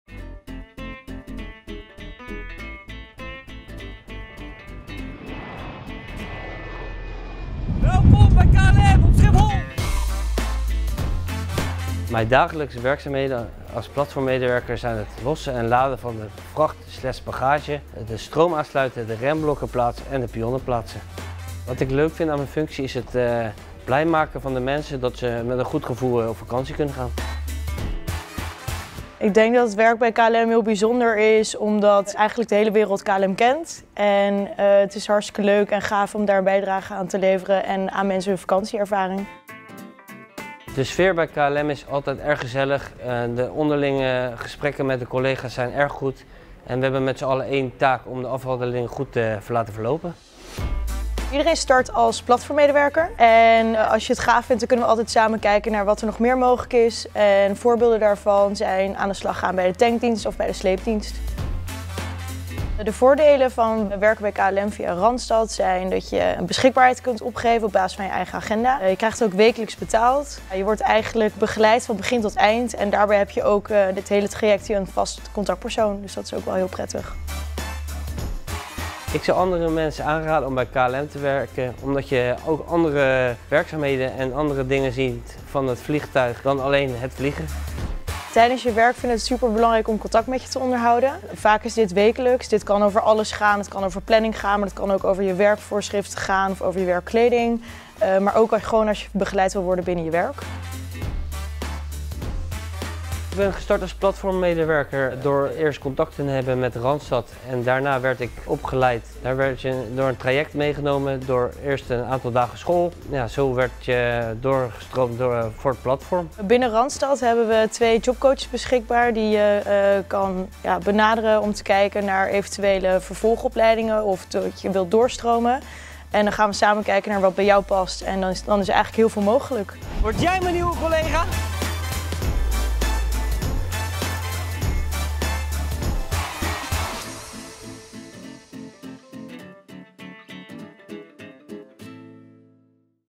Werken bij KLM via Randstad
Hoe is het om te werken als  platformmedewerker bij KLM op Schiphol? Sander vertelt je er alles over.

00:00 introductie
00:11 dagelijkse werkzaamheden
00:22 positieve aspecten van het werk
00:40 werksfeer
00:50 doorgroeimogelijkheden
1:02 voordelen van werken via Randstad
1:18 waarom werken als platformmedewerker?
1:43 hoe word je platformmedewerker?
1:56 waarvoor kun je terecht bij de jobcoaches van Randstad?